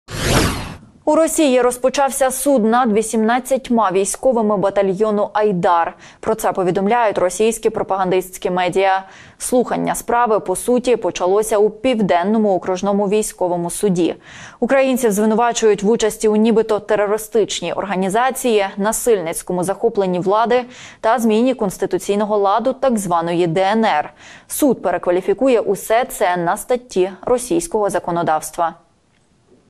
У росії розпочався суд над 18 військовими батальйону «Айдар»
У росії розпочався суд над 18 військовими батальйону «Айдар». Про це повідомляють російські пропагандистські медіа. Слухання справи по суті почалося в Південному окружному військовому суді. Українців звинувачують в участі в нібито терористичній організації, насильницькому захопленні влади та зміні конституційного ладу так званої «ДНР». Суд перекваліфікує усе це на статті російського законодавства.